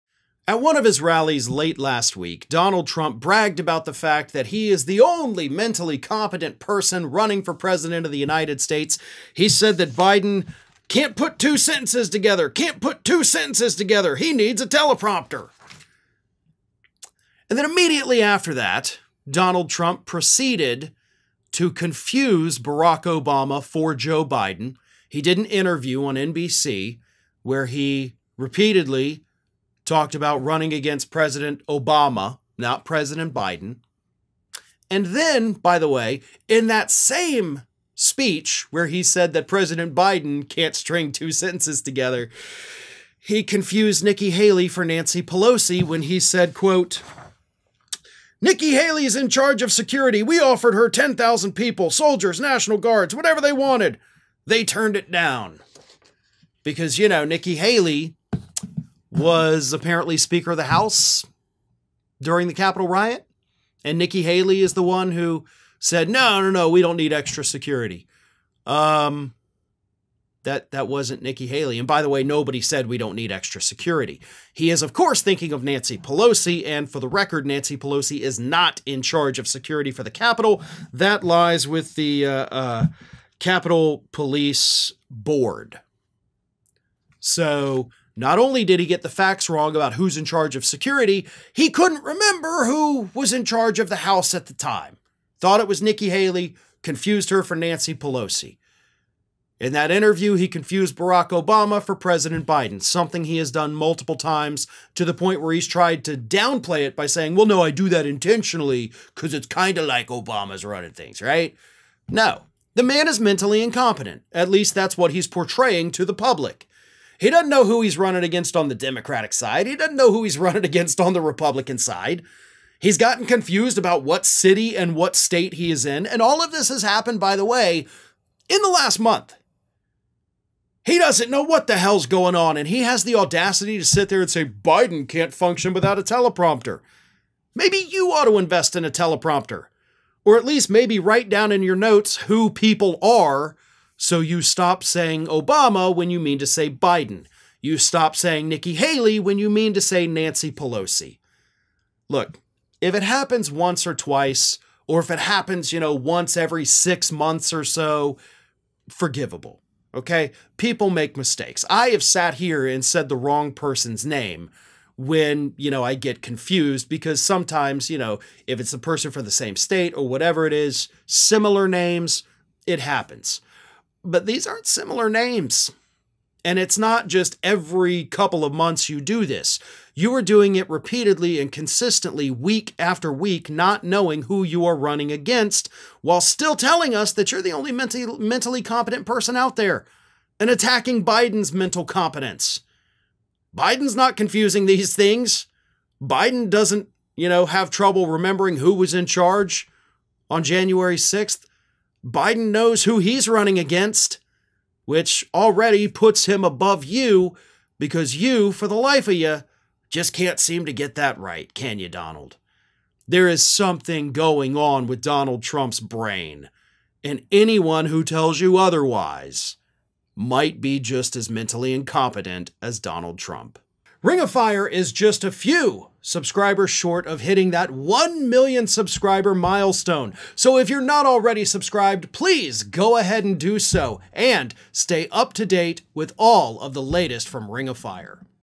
Mentally Incompetent Trump Confuses Nikki Haley For Nancy Pelosi
Donald Trump has no idea who he is running against. In the span of 24 hours late last week, Trump confused Barack Obama for Joe Biden in an interview and then he confused Nikki Haley for Nancy Pelosi when talking about the capitol riot. This is happening very frequently, and the irony is that Trump continues to tout his mental prowess (including at a rally last week) and he acts like he's the only competent person running for president. Nothing could be further from the truth, as Ring of Fire's Farron Cousins explains.

At one of his rallies. Late last week, Donald Trump bragged about the fact that he is the only mentally competent person running for President of the United States. He said that Biden can't put two sentences together, can't put two sentences together. He needs a teleprompter. And then immediately after that, Donald Trump proceeded to confuse Barack Obama. For Joe Biden. He did an interview on NBC, where he repeatedly talked about running against President Obama, not President Biden. And then, by the way, in that same speech where he said that President Biden can't string two sentences together, he confused Nikki Haley for Nancy Pelosi, when he said, quote, Nikki Haley's in charge of security. We offered her 10,000 people, soldiers, national Guards, whatever they wanted, they turned it down because, you know, Nikki Haley was apparently speaker of the house during the Capitol Riot. And Nikki Haley is the one who said, no, no, no, we don't need extra security.

Um, that that wasn't Nikki Haley. And by the way, nobody said, we don't need extra security. He is of course, thinking of Nancy Pelosi. And for the record, Nancy Pelosi is not in charge of security for the Capitol that lies with the, uh, uh, Capitol Police Board. So not only did he get the facts wrong about who's in charge of security, he couldn't remember who was in charge of the house at the time. Thought it was Nikki Haley confused her for Nancy Pelosi in that interview. He confused Barack Obama for President Biden, something he has done multiple times to the point where he is tried to downplay it by saying, well, no, I do that intentionally. 'cause it's kind of like Obama's running things, right? No, the man is mentally incompetent. At least that's what he's portraying to the public. He doesn't know who he's running against on the Democratic side.

He doesn't know who he's running against. On the Republican side, he's gotten confused about what city and what state he is in. And all of this has happened, by the way, in the last month. He doesn't know what the hell's going on. And he has the audacity to sit there and say, Biden can't function without a teleprompter. Maybe you ought to invest in a teleprompter, or at least maybe write down in your notes who people are. So you stop saying Obama, when you mean to say Biden, you stop saying Nikki Haley, when you mean to say Nancy Pelosi. Look, if it happens once or twice, or if it happens, you know, once every six months or so, forgivable, okay? People make mistakes. I have sat here and said the wrong person's name when you know, I get confused because sometimes, you know, if it's the person for the same state or whatever it is, similar names, it happens. But these aren't similar names. And it's not just every couple of months you

Do this, you are doing it repeatedly and consistently week after week, not knowing who you are running against while still telling us that you're the only mental, mentally competent person out there and attacking Biden's mental competence. Biden's not confusing these things. Biden doesn't, you know, have trouble remembering who was in charge on January 6th. Biden knows who he's running against, which already puts him above you because you, for the life of, you just can't seem to get that right. Can you, Donald?